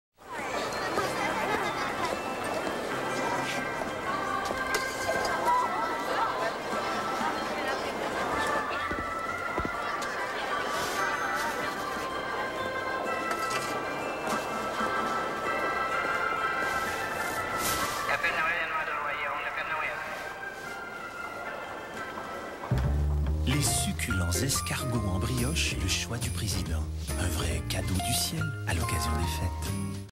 Le Choix du Président
Publicité Le Choix du Président avec Claude Blanchard